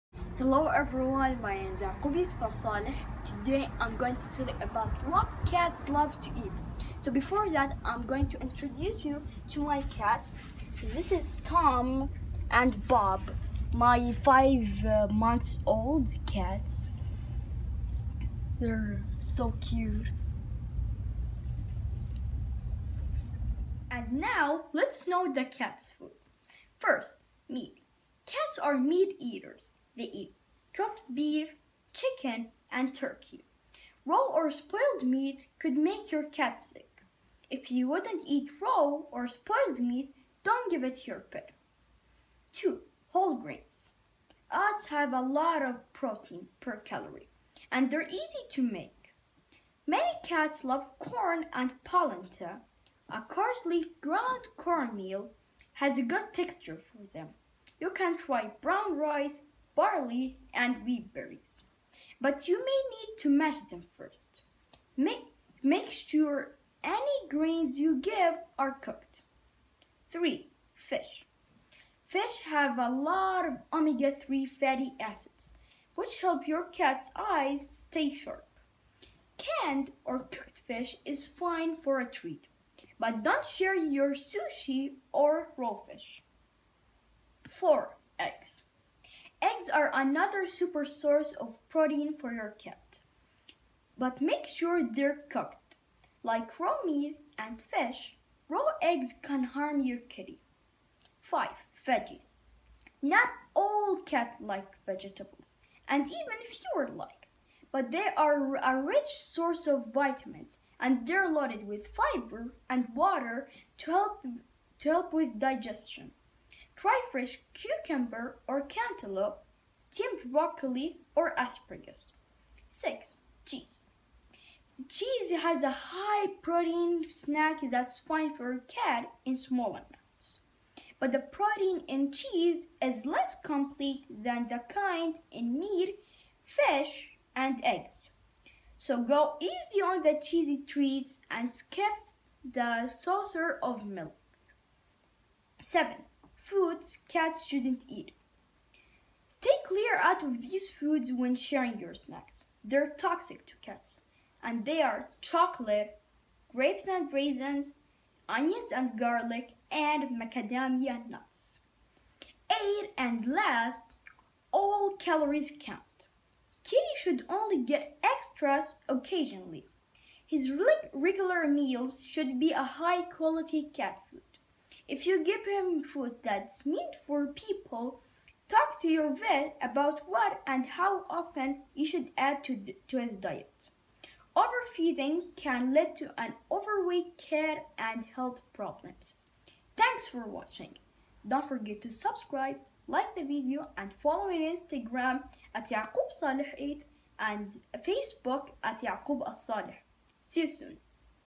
Raising cats Chapter 2: What are cats foods
Many things about cats foods you have to learn and to know..
Follow me in my instagram account at yacoubsaleh8,
And my Facebook account at Yacoub Al Saleh.